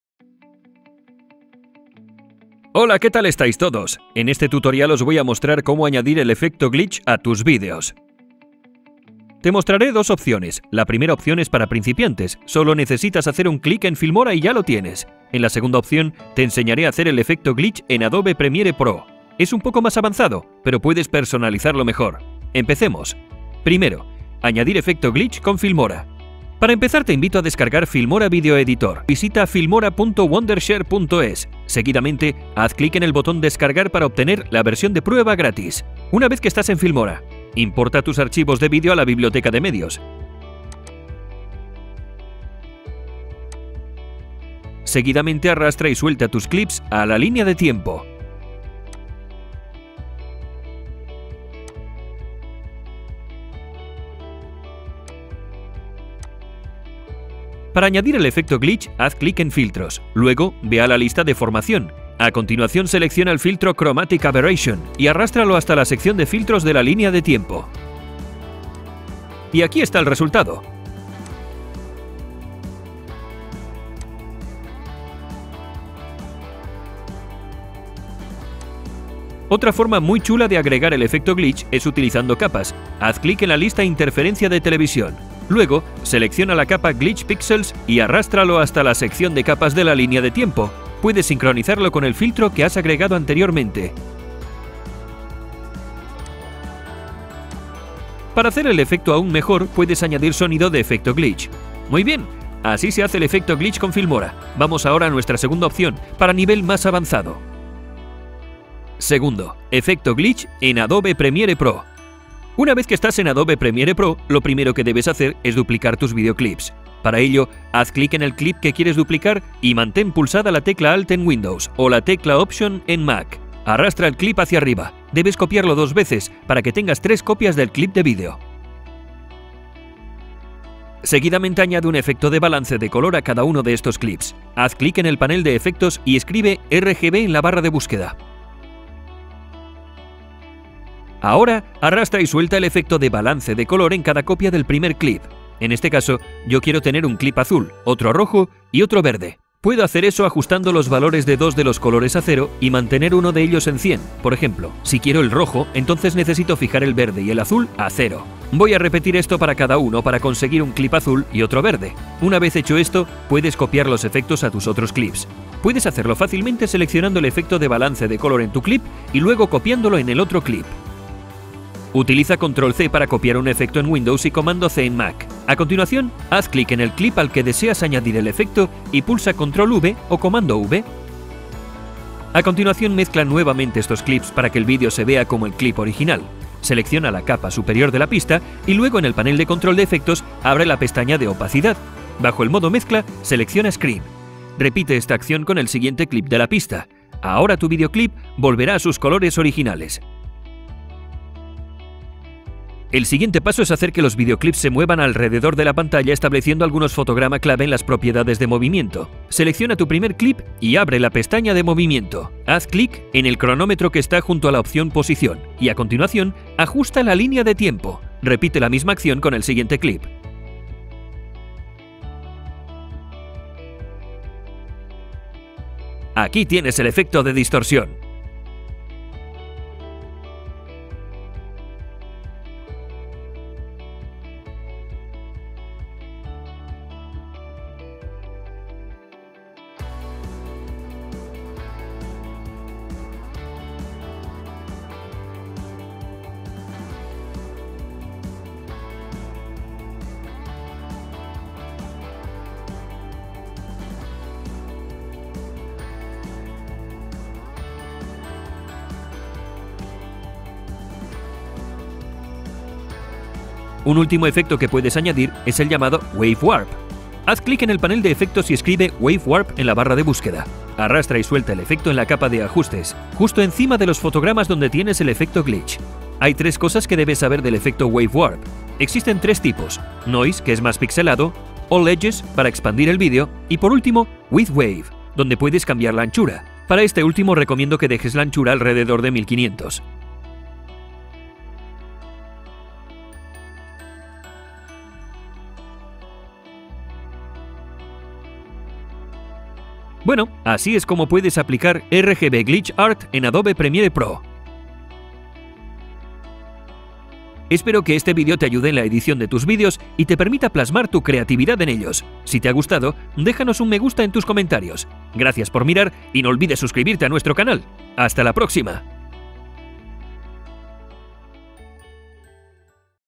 ¿Cómo añadir Efecto Glitch? – Añadir Fallos y Distorsión a un vídeo
1. Primero. Añadir Efecto Glitch con Filmora.

Para empezar, te invito a Descargar Filmora Video Editor. Haz clic en el enlace que te dejo en la caja de descripción, o visita filmora.wondershare.es. Seguidamente, haz clic en el botón descargar para obtener la versión de prueba gratis.

Una vez que estás en Filmora:
Importa tus archivos de vídeo a la biblioteca de medios.
Seguidamente, arrastra y suelta tus clips a la línea de tiempo.
Para añadir el Efecto Glitch, haz clic en Filtros. Luego, ve a la lista Deformación. A continuación, selecciona el filtro Chromatic Aberration, y arrástralo hasta la sección de Filtros de la línea de tiempo.

Otra forma muy chula de agregar el Efecto Glitch es utilizando Capas. Haz clic en la lista Interferencia de TV. Luego, selecciona la capa Glitch Pixels, y arrástralo hasta la sección de capas de la línea de tiempo. Puedes sincronizarlo con el filtro que haz agregado anteriormente.

2. Segundo. Efecto Glitch en Adobe Premiere Pro.

Lo primero que debes hacer es duplicar tus vídeo clips. Para ello, haz clic en el clip que quieres duplicar y mantén pulsada la tecla Alt en Windows, o la tecla Option en Mac. Arrastra el clip hacia arriba. Debes copiarlo dos veces para que tengas tres copias del clip de vídeo.

Seguidamente, añade un efecto de balance de color a cada uno de estos clips. Haz clic en el panel de efectos y escribe RGB en la barra de búsqueda. Ahora, arrastra y suelta el efecto de balance de color en cada copia del primer clip. En este caso yo quiero tener un clip azul, otro rojo y otro verde. Puedo hacer eso ajustando los valores de dos de los colores a cero y mantener uno de ellos en 100. Por ejemplo, si quiero el rojo entonces necesito fijar el verde y el azul a cero. Voy a repetir esto para cada uno para conseguir un clip azul y otro verde. Una vez hecho esto, puedes copiar los efectos a tus otros clips. Puedes hacerlo fácilmente seleccionando el efecto de balance de color en tu clip y luego copiándolo en el otro clip.

 Utiliza Control + C para copiar un efecto en Windows y Comando + C en Mac. A continuación, haz clic en el clip al que deseas añadir el efecto y pulsa Control + V o Comando + V.

A continuación, mezcla nuevamente estos clips para que el vídeo se vea como el clip original. Selecciona la capa superior de la pista y luego en el panel de control de efectos, abre la pestaña de opacidad. Bajo el modo Mezcla, selecciona Screen. Repite esta acción con el siguiente clip de la pista. Ahora tu videoclip volverá a sus colores originales.

 El siguiente paso es hacer que los videoclips se muevan alrededor de la pantalla estableciendo algunos fotogramas clave en las propiedades de movimiento. Selecciona tu primer clip y abre la pestaña de movimiento. Haz clic en el cronómetro que está junto a la opción Posición y, a continuación, ajusta la línea de tiempo. Repite la misma acción con el siguiente clip.

Un último efecto que puedes añadir es el llamado Wave Warp. Haz clic en el panel de efectos y escribe Wave Warp en la barra de búsqueda. Arrastra y suelta el efecto en la Capa de ajustes, justo arriba de los fotogramas donde tienes el Efecto Glitch. Hay tres cosas que debes saber del efecto Wave Warp. Existen tres tipos: Noise, que es más pixelado; All Edges, para expandir el vídeo; y por último Width Wave, dónde puedes cambiar la anchura. Para este último recomiendo que dejes la anchura alrededor de 1500.
Bueno, así es como puedes aplicar Rgb Glitch Art en Adobe Premiere Pro.